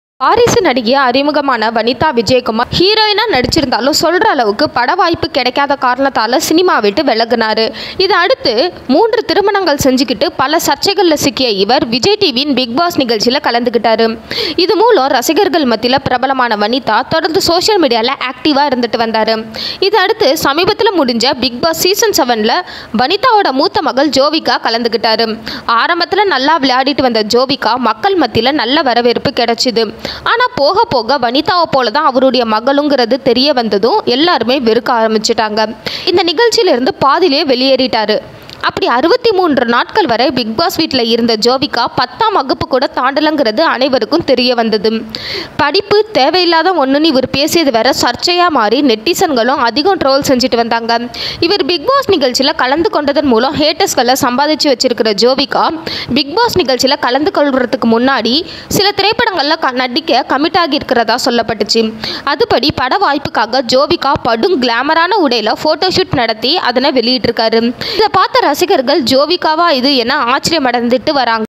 ரம்பா என்னப்பா ரம்பா பிக் பாஸ் ஜோவிகாவா பாருங்க.. ஆத்தாடி இம்புட்டு கிளாமர் ஆகாதும்மா..
அதன்படி பட வாய்ப்புகளுக்காக ஜோவிகா படும் கிளாமரான உடையில் போட்டோ ஷூட் நடத்தி அதனை வெளியிட்டுள்ளார். இதனை பார்த்த ரசிகர்கள் ஜோவிகாவா இது என ஆச்சரியமடைந்து வருகின்றனர்.